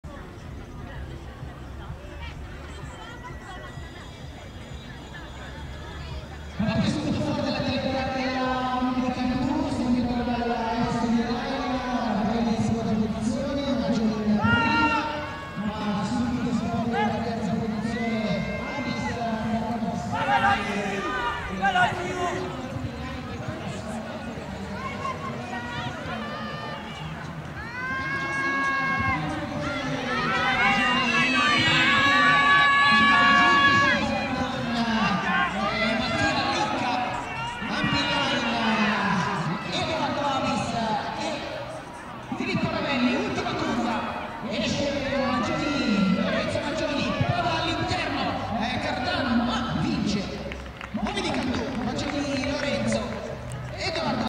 Campionati Interprovinciali Strada 2024 A-J-S-M - A/M 1 giro sprint, finali
Campionati Interprovinciali 2024, Pattinaggio Corsa, specialità "strada".
17 Marzo 2024, Mariano Comense (CO) - Italy.
Categoria Allievi Maschile.
1 giro sprint, finali.

Risultati
1 31 MAGGIONI LORENZO 651 G.P.MOBILI CANTU' 46.439
2 54 ABIS EDOARDO 3370 CARDANO SKATING 46.608
3 50 RICCA MATTIA 3814 ANPINLINE 46.699
4 93 RAVELLI FILIPPO 4000 ASD INLINE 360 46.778